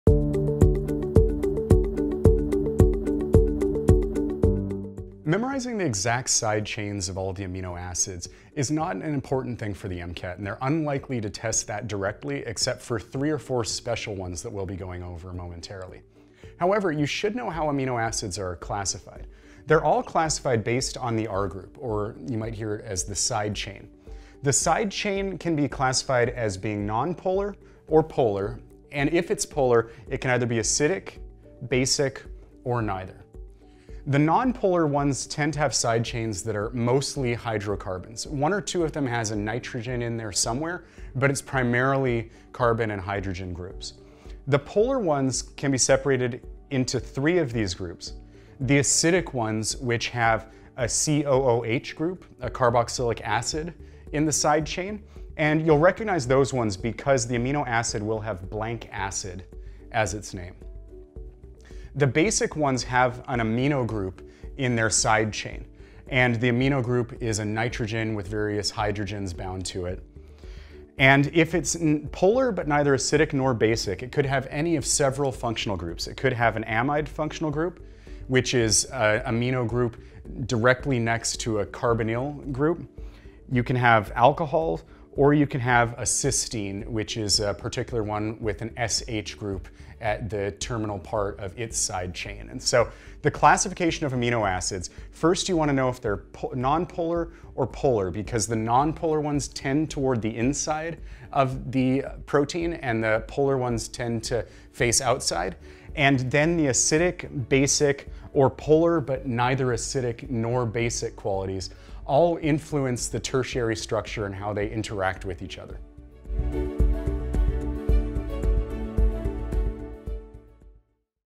Amino Acids: Polar vs Non-Polar, Acidic, Basic, and Neutral
Amino Acids 2 : Classification